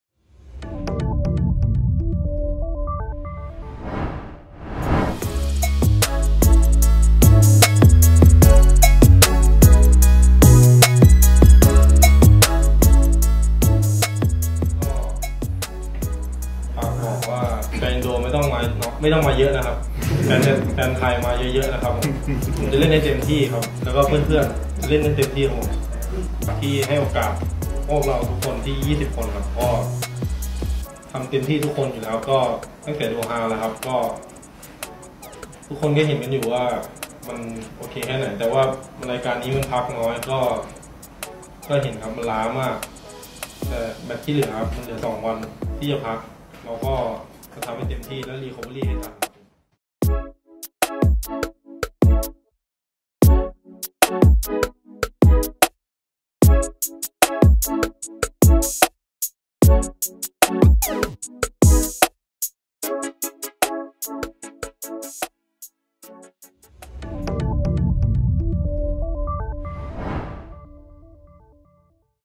Jelang Final SEA Games, Pemain Timnas Thailand Beri Pesan ke Fans Indonesia | tvOne Minute
Jakarta, - Jelang Final SEA Games, Pemain Timnas Thailand Beri Pesan ke Fans Indonesia | tvOne Minute

Salah satu pemain timnas Thailand U-22 Songchai Thongcham memiliki pesan yang cukup menarik perhatian suporter Indonesia jelang laga final SEA Games 2023. Ia mengatakan kepada fans Timnas Indonesia tidak usah datang saat final SEA Games 2023 nanti.

TM01